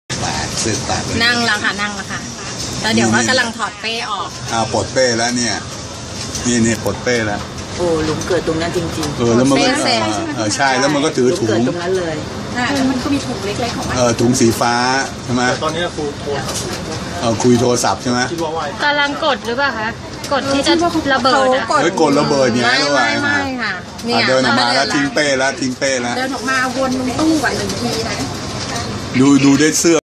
ชายต้องสงสัย มือวางระเบิด BKK Bomb suspected 17/08/2015
คลิปชายเสื้อเหลือง (yellow shirt, Suspected was drop a bomb) ซึ่งเป็นผู้ต้องสงสัยวางเป้ระเบิดศาลพระพรหม ณ ราชประสงค์ ประมาณ 19.00 น. BKK Bomb suspect วันที่ 17/08/2558